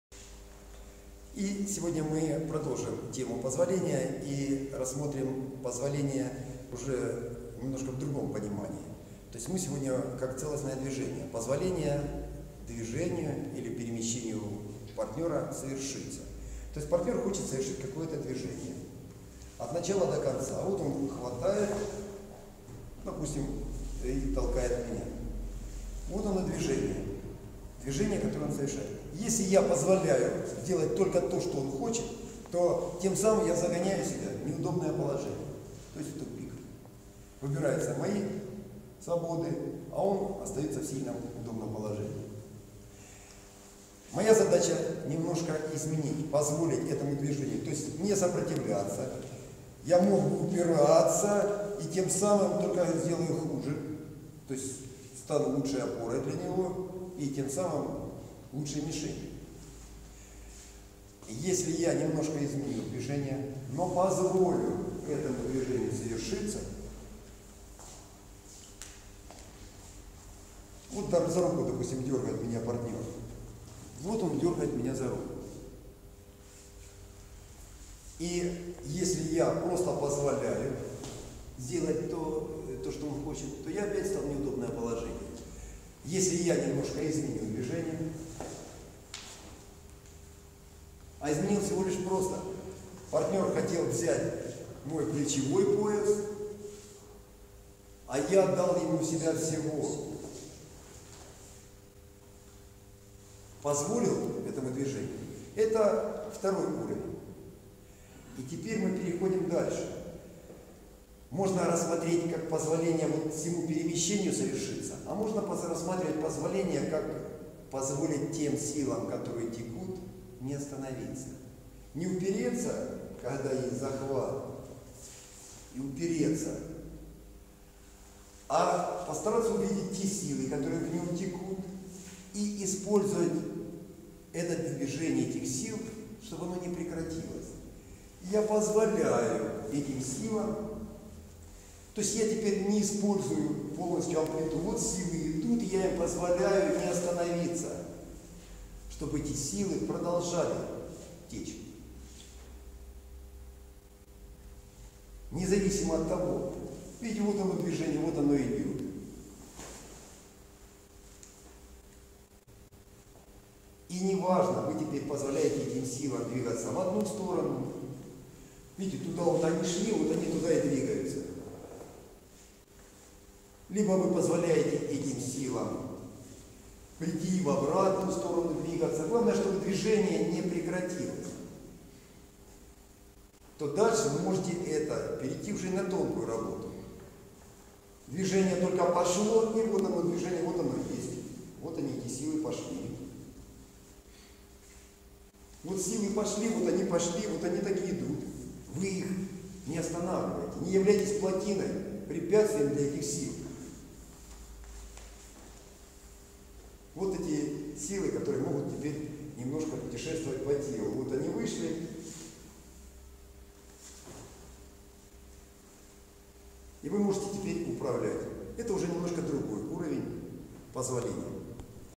Занятие №22 Александр Руденко. Позволение Ч. 2 Рукопашный бой. Казачий Спас. Харьков
Занятия в клубе рукопашного боя "Казачий Спас".Харьков
Видеоуроки и еще много интересного видео и информации по Казачьему Спасу и Русскому рукопашному бою в группе ВК Клуб рукопашного Боя Казачий спас . Харьков и в группе